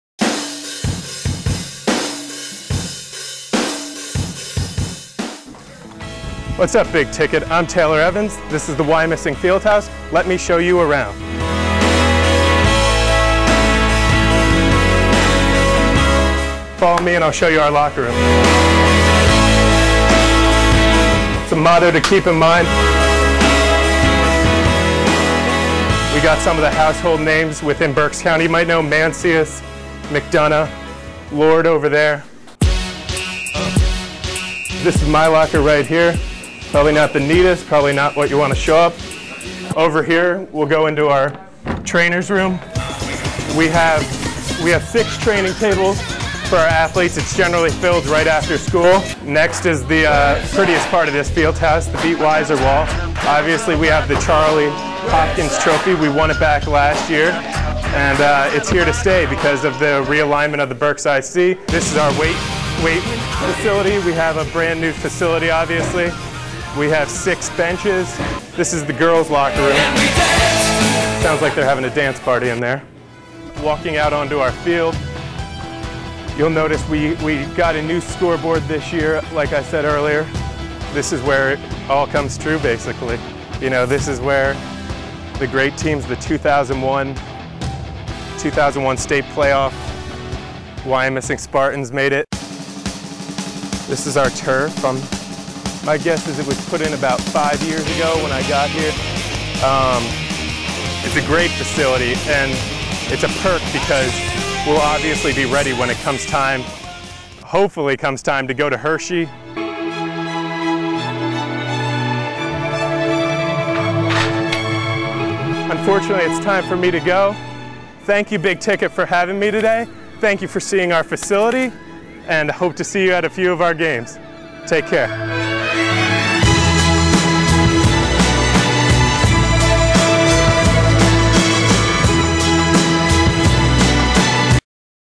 Wyo Field House
A walk through of the field house for a local high school.